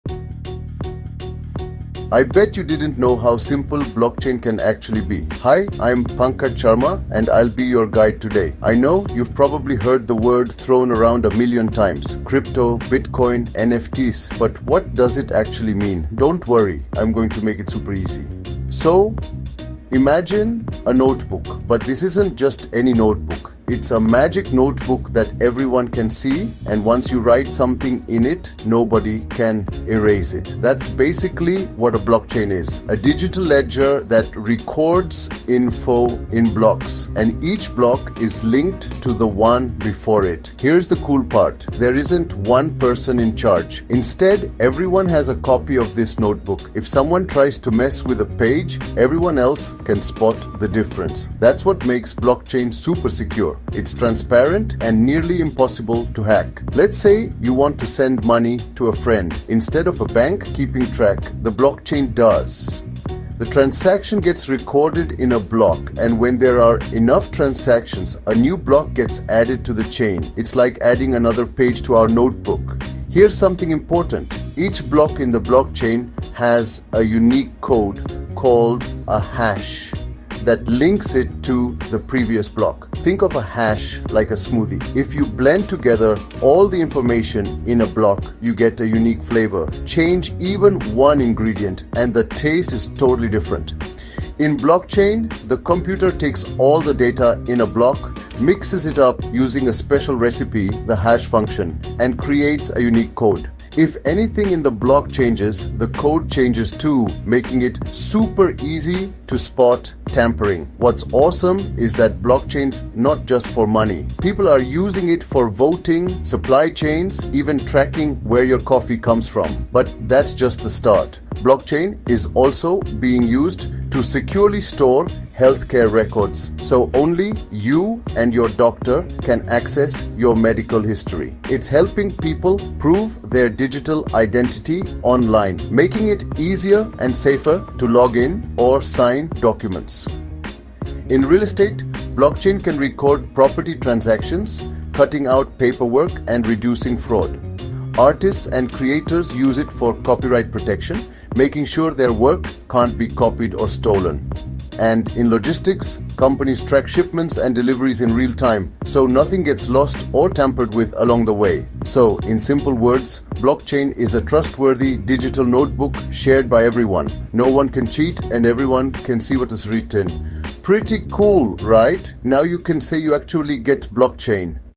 Blockchain Simplified: How It Works!
Dive into the fascinating world of blockchain technology with our video, "Blockchain Simplified:

How It Works!" In this easy-to-understand guide, we break down the complexities of blockchain into simple terms.

Discover how this revolutionary technology operates, its key components, and real-world applications that are transforming industries.

Whether you're a beginner or just curious about blockchain, this video provides the essential knowledge you need to grasp how it works.